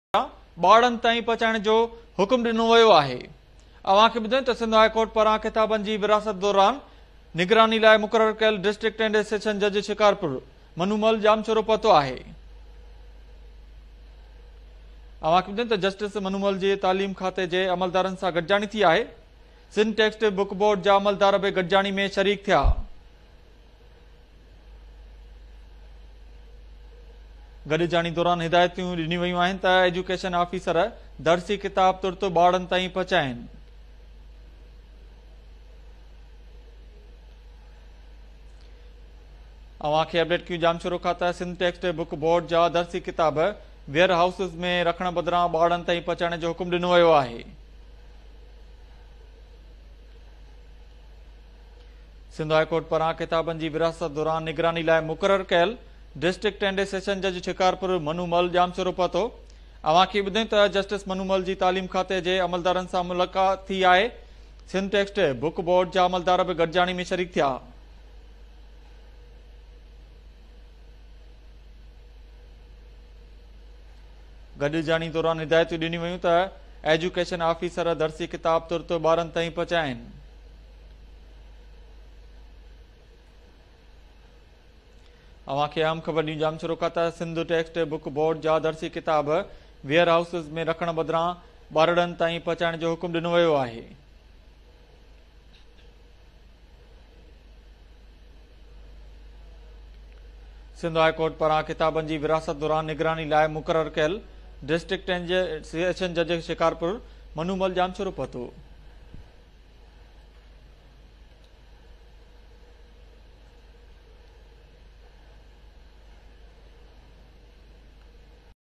BREAKING NEWS Jamshoro: Sindh Textbook Board ordered to deliver textbooks to children
BREAKING NEWS Jamshoro: Sindh Textbook Board ordered to deliver textbooks to children instead of keeping them in warehouses